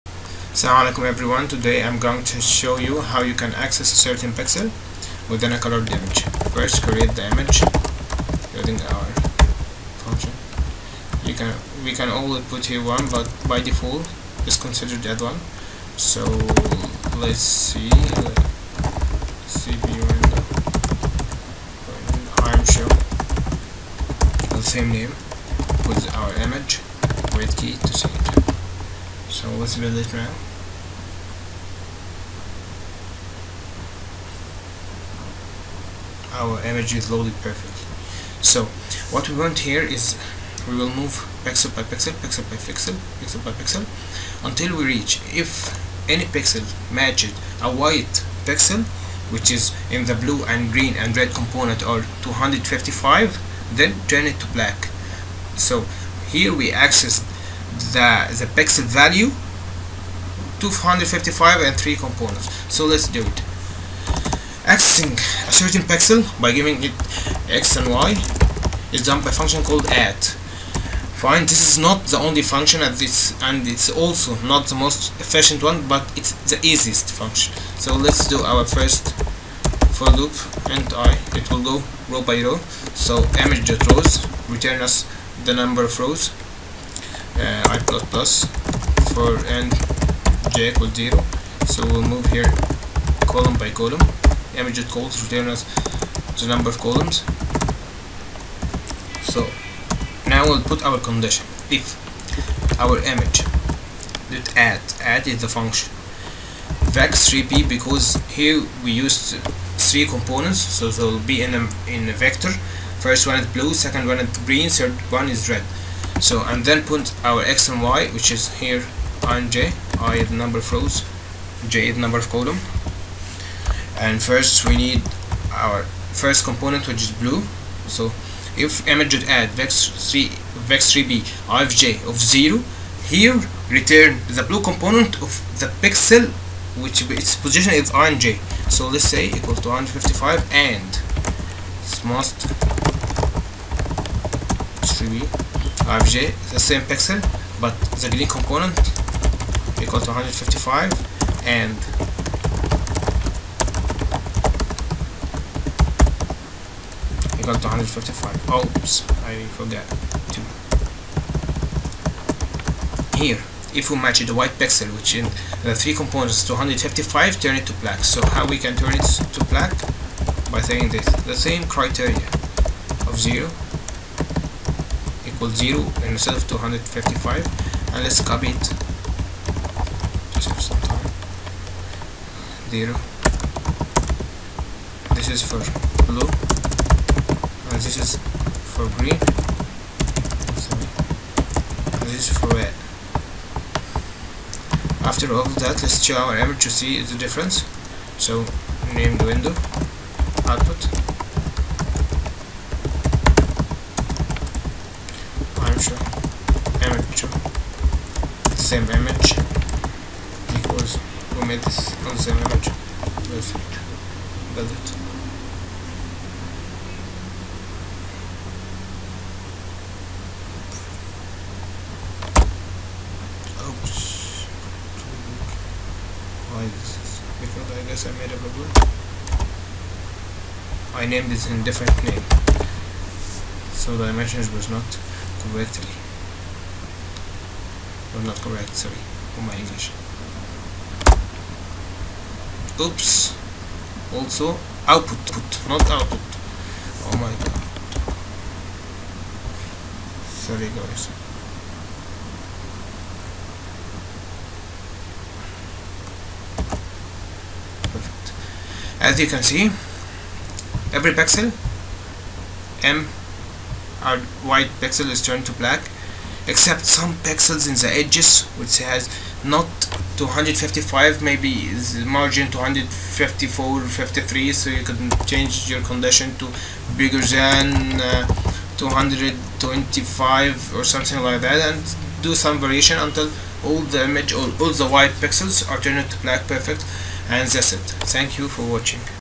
Lesson-5: Accessing pixels in colored images using openCV.
Welcome to the 5th lesson in the OpenCV tutorials using c++